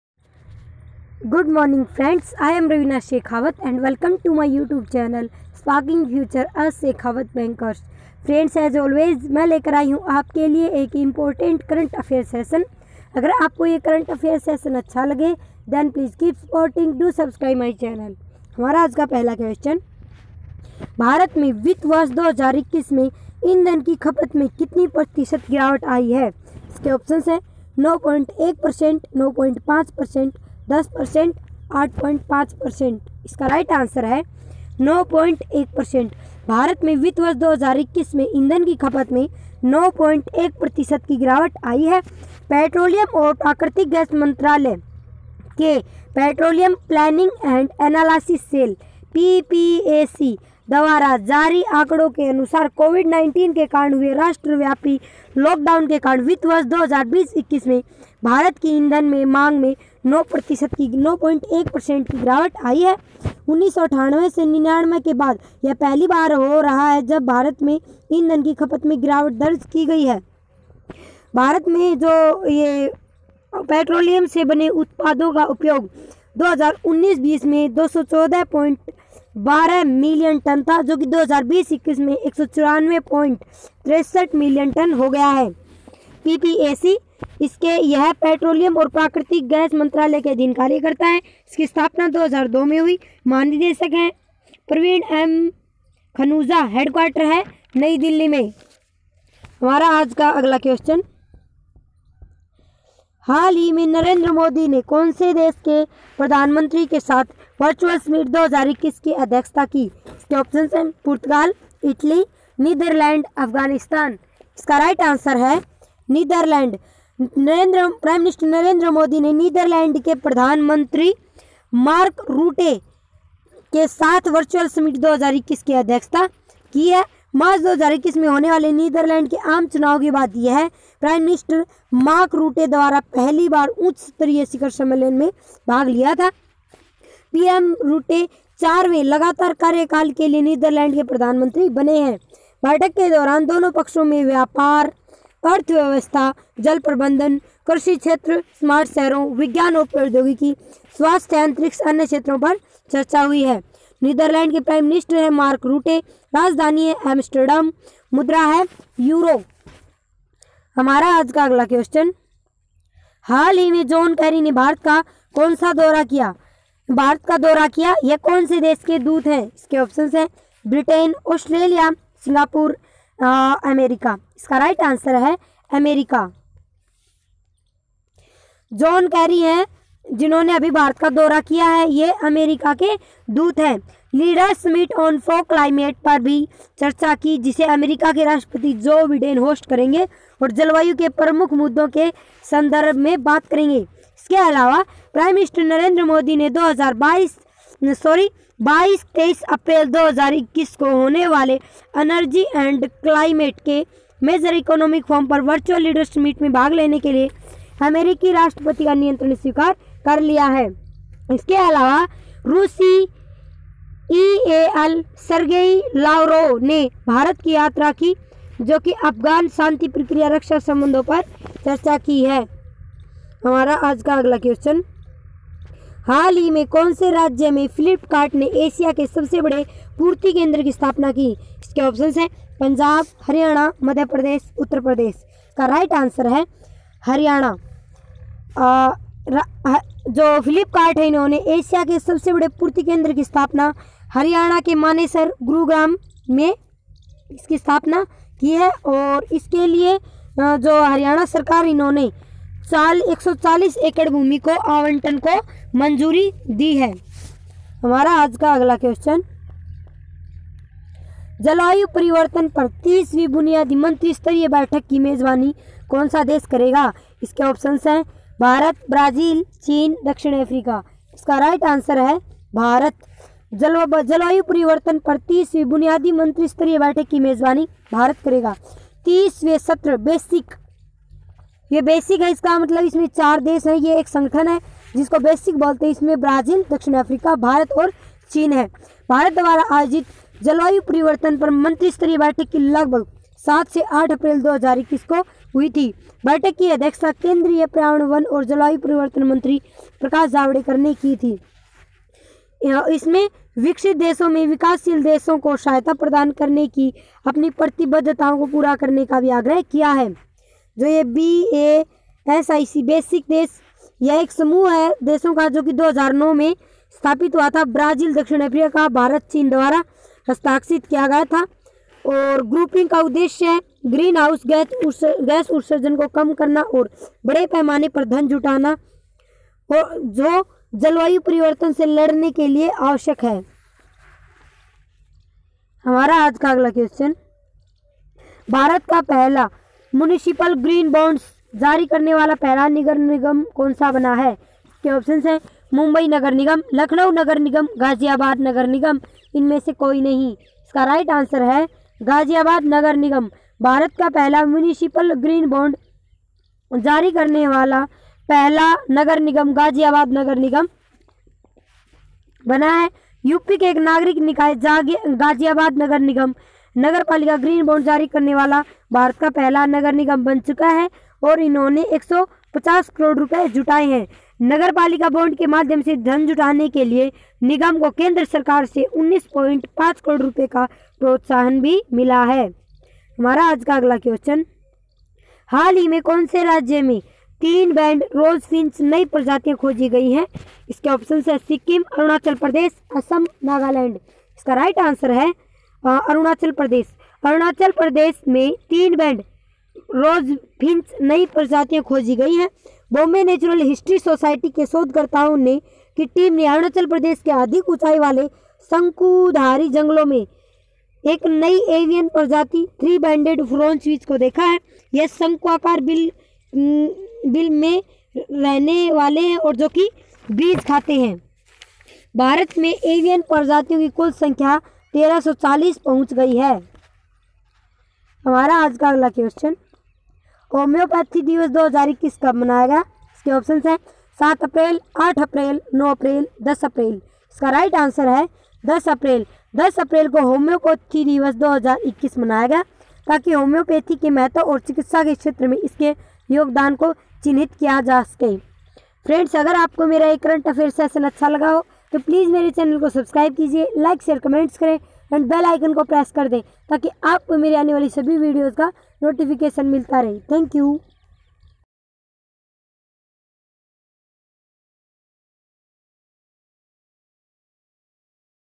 7am 13 April 2021 daily current affairs for bank SSC other exams Sparking Future Sshekhawat bankers
Daily current Affairs 2021 by Ravina shekhawat current Affairs | Daily current Affairs in hindi| Most important current Affairs in hindi

My current Affairs questions help you to compete Bank exam,ssc,railway nd other exams